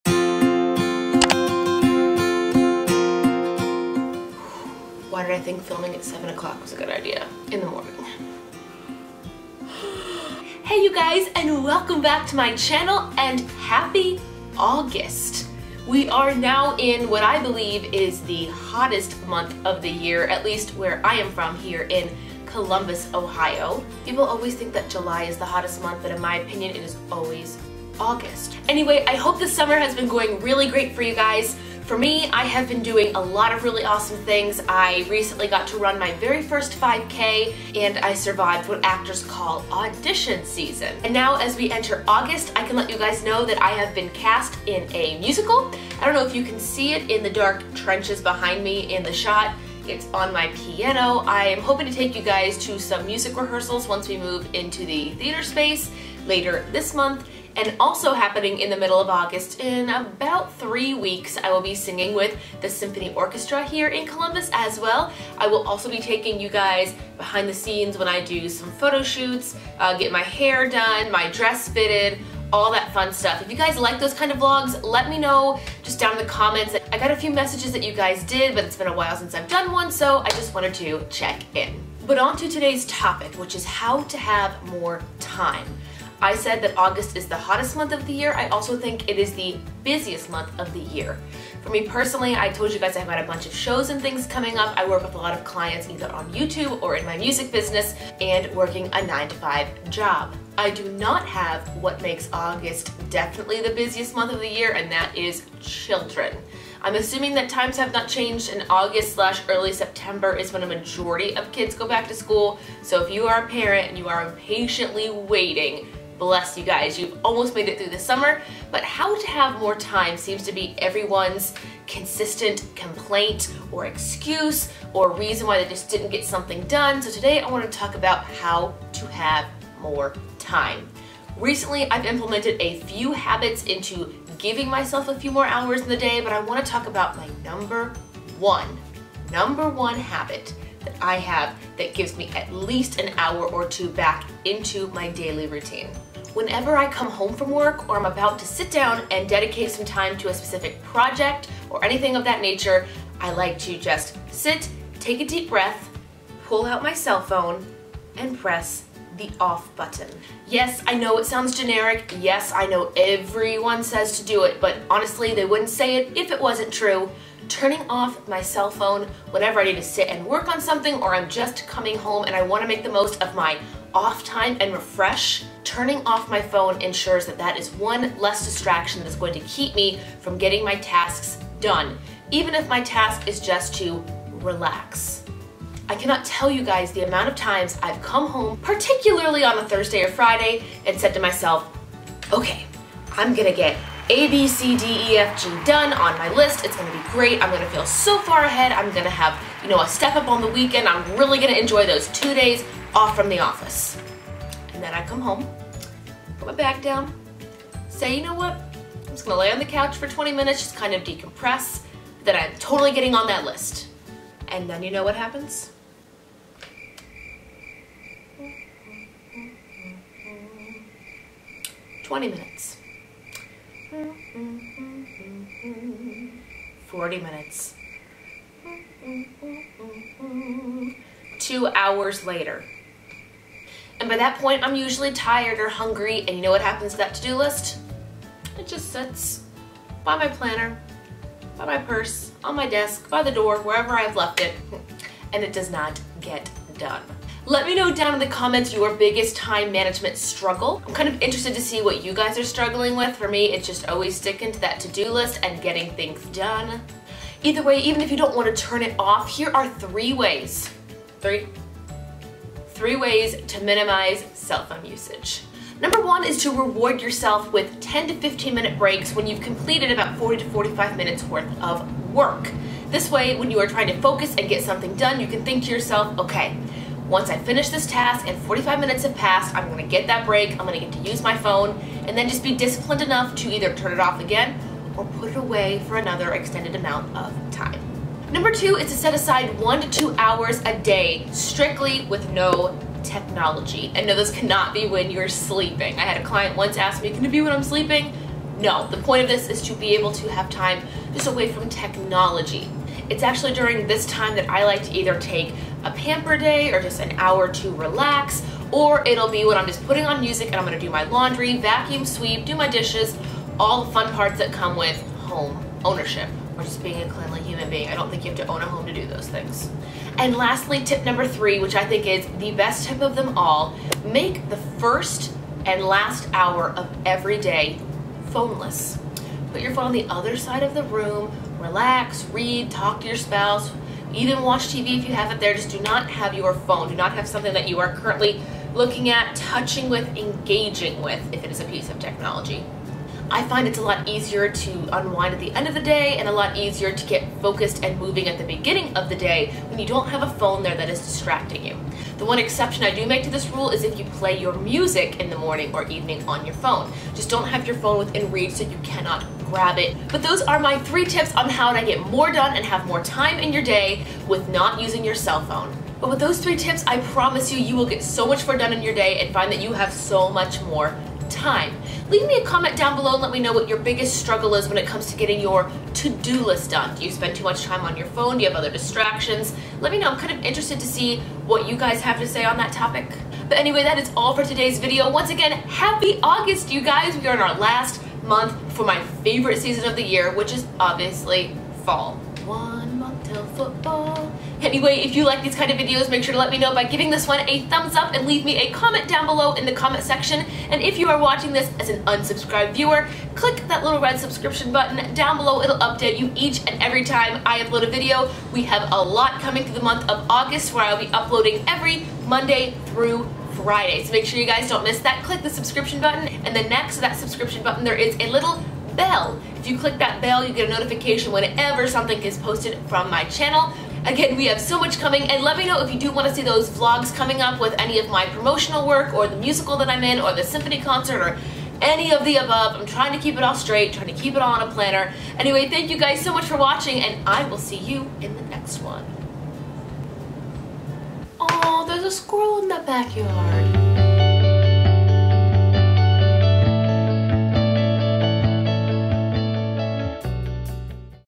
Top Time Saving Tips - How To Have More Time
Ironically, I'm writing this description box last minute when I have very little time! None-the-less, here are my top tips to find more time in your day and get more important tasks done!

✉ Mail Time  ✉
Kristen Marie

▪Vlog Like A Boss ($14)

🎥 Requested Videos 🎥